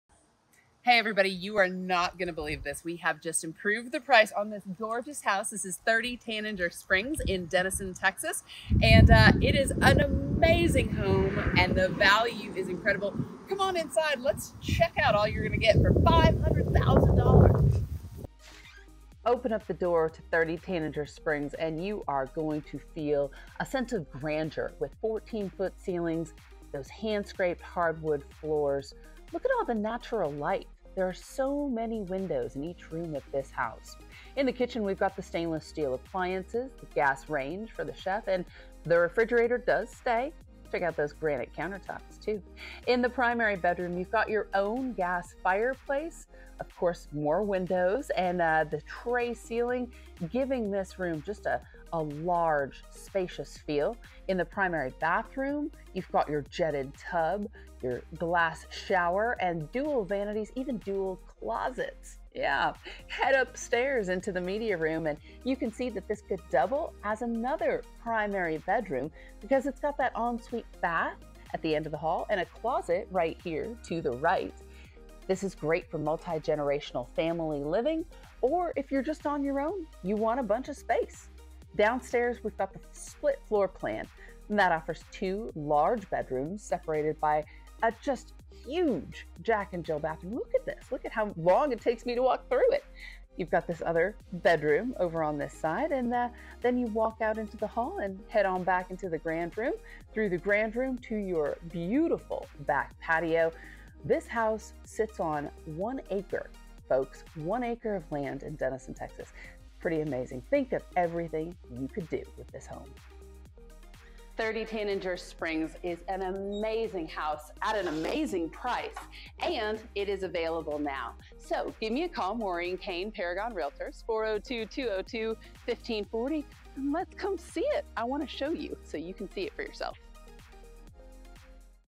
Luxury Living on One Acre in Denison, Texas
Luxury living awaits in this gorgeous custom home that sits on one acre in a gated community!  Check out the finishes on 30 Tananger Springs, Denison, TX, and imagine yourself curling up in front of a cozy fire in your primary bedroom on those chilly fall nights.  Step out onto the back patio for a beautiful view with your morning coffee.  Picture a gathering with friends in the warm and welcoming living area.  Then call your favorite Realtor® to make it your own!